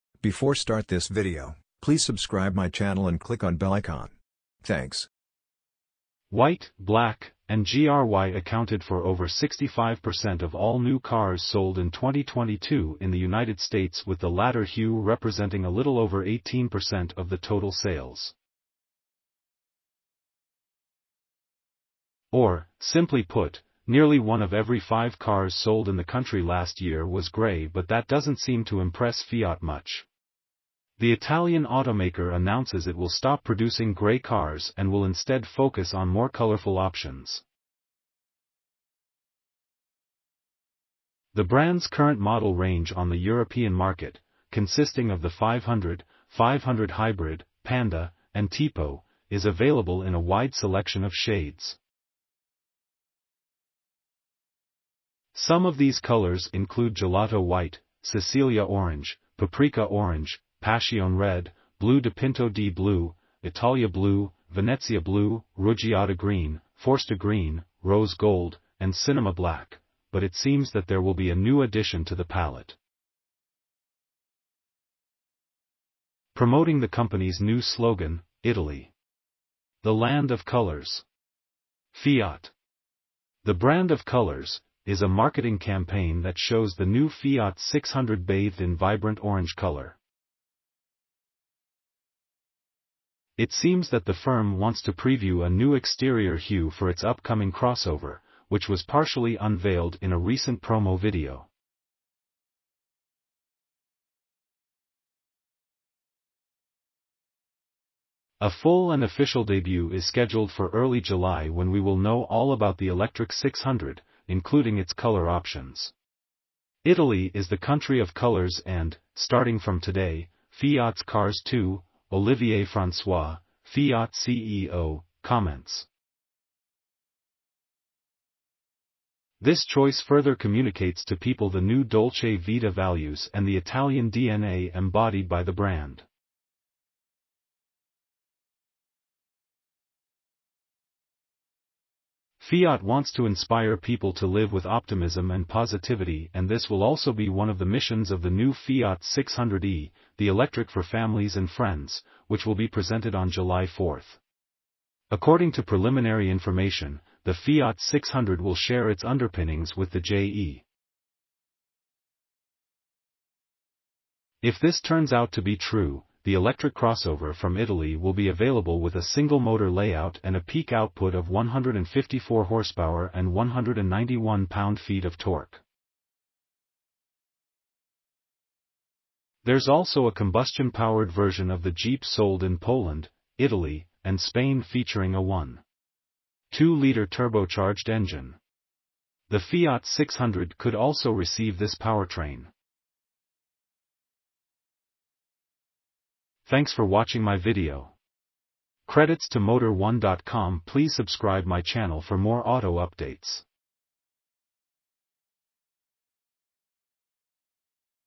Fiat Says Goodbye To Boring Gray, Bathes 600e In Orange Paint
White, black, and grаy accounted for over 65 percent of all new cars sold in 2022 in the United States with the latter hue representing a little over 18 percent of the total sales. Or, simply put, nearly one of every five cars sold in the country last year was gray but that doesn’t seem to impress Fiat much. The Italian automaker announces it will stop producing gray cars and will instead focus on more colorful options. The brand’s current model range on the European market – consisting of the 500, 500 Hybrid, Panda, and Tipo – is available in...